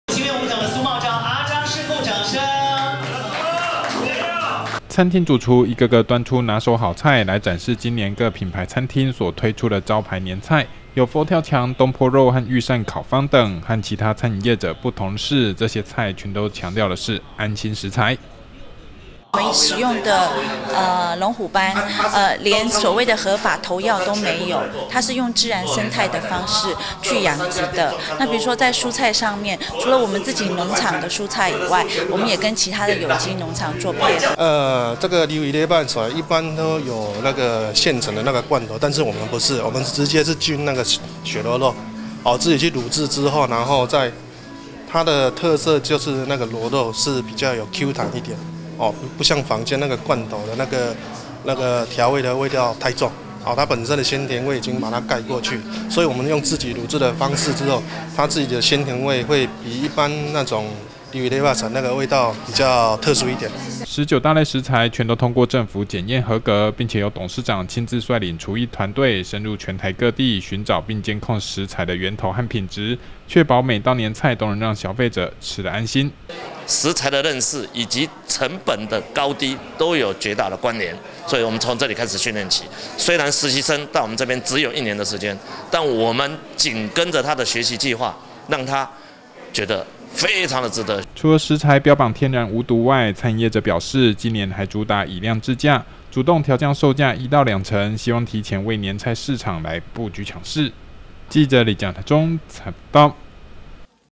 大台中新聞-新月集團發表安心年菜
去年,毒澱粉.毒醬油和假油等議題,延燒一整年,食品安全,愈來愈受到消費者重視,為此,一家餐飲集團今年推出的年菜組合,就以安心為訴求,所選用的雞鴨魚肉和蔬菜,甚至油鹽醋茶等,全都標榜政府檢驗合格,並且還親自率領廚藝團隊,監控食材源頭和品質,希望藉此,提前搶攻年菜市場商機「以上新聞可從哈TV APP收視」